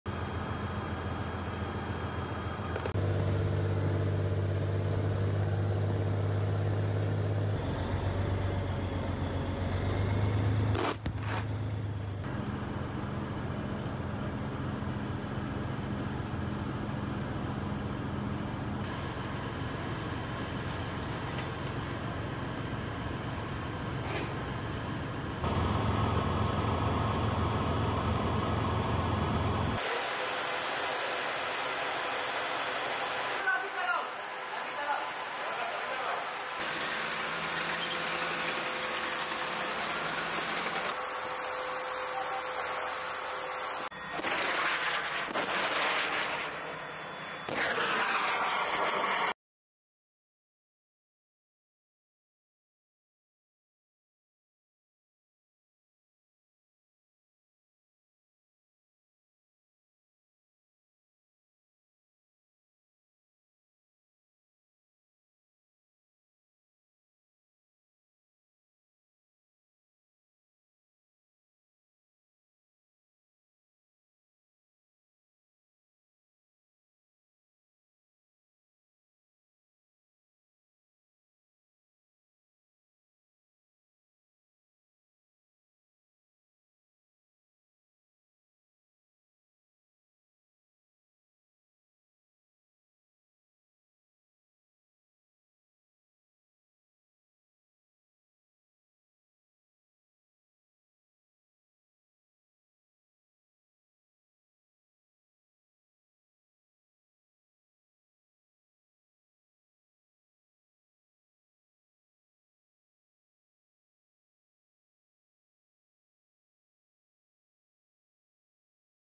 Israel’s D9R modified bulldozers and armoured ‘Teddy Bear’ tanks lead ground operations in Gaza
Israeli forces released footage on Sunday of military engineering machines operating in the Gaza Strip.

The footage showed heavy bulldozers operating in the Gaza Strip, with another footage showing tanks operating in the area as part of its expanded ground operations.

Israel expanded its ground operation in Gaza on Saturday (October 28), sending in tanks and infantry backed by massive strikes from the air and sea.

Israeli Prime Minister Benjamin Netanyahu said Saturday that the second stage of the war against Hamas has begun, with the goal of destroying the group's armed wing and government and returning hostages held in Gaza home.